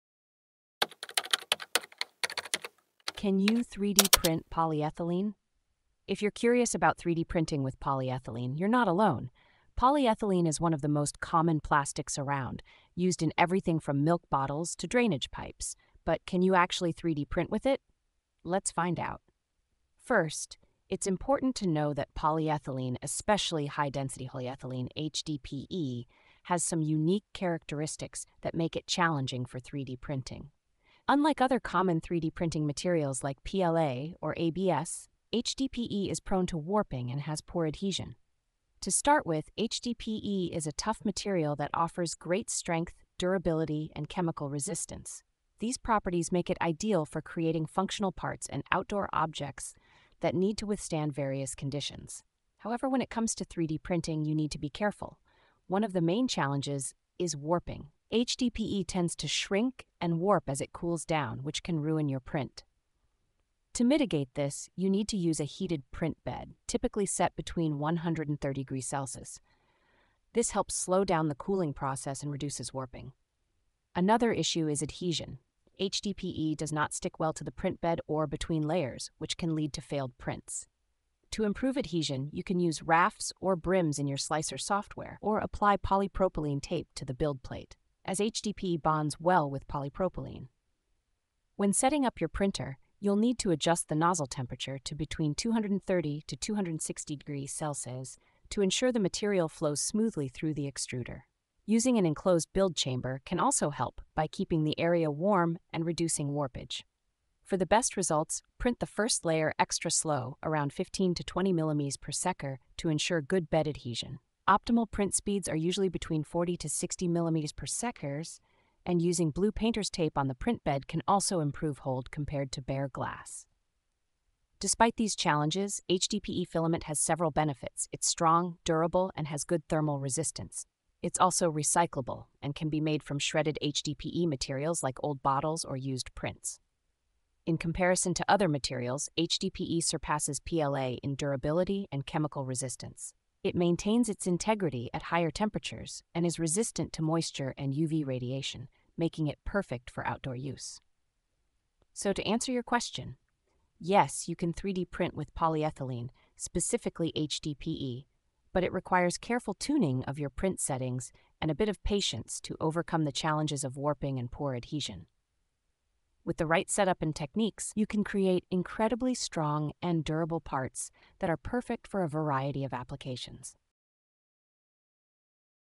Can You 3D Print Polyethylene? - Chemistry For Everyone
Can You 3D Print Polyethylene? If you've ever been curious about the potential of 3D printing with polyethylene, you're in the right place. In this video, we will discuss the fascinating world of 3D printing using high-density polyethylene (HDPE). This material is widely used in everyday products, but its properties make it a unique challenge for 3D printing enthusiasts.

We will cover the essential aspects of working with HDPE, including the challenges of warping and adhesion that can affect print quality. You'll learn about the optimal settings for your 3D printer, including temperature adjustments and the importance of a heated print bed. We will also share practical tips for improving adhesion to ensure your prints turn out successfully.

Additionally, we’ll highlight the benefits of using HDPE filament, such as its strength, durability, and recyclability. You’ll see how HDPE compares to other common materials like PLA, especially in terms of performance in various environments.